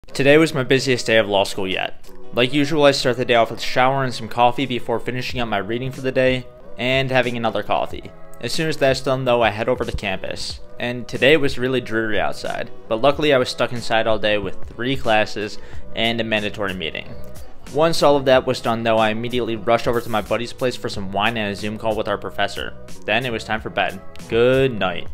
My Busiest Day At Law School Yet
Hope you all enjoyed the short! This short detailed a day in my life as a busy law school student. I am currently a 1L law school student at the Sandra Day O'Connor College of Law at Arizona State University. This was my busiest day of law school yet! I had 3 different classes, a mandatory meeting, and office hours at night with one of my professors. I started the day off with some reading for my property law class. After that I headed into downtown Phoenix for class at the ASU downtown campus. While there, I had my legal advocacy class, property law, and professional responsibility. I also had a mandatory meeting about signing up for classes for the fall semester. After that was all done I rushed over to my friends place for some wine and a zoom call with my criminal law professor. My crim law professor does office hours at night where we get to have drinks with him and hang out. It was a super busy day but also a lot of fun! What does your daily schedule look like?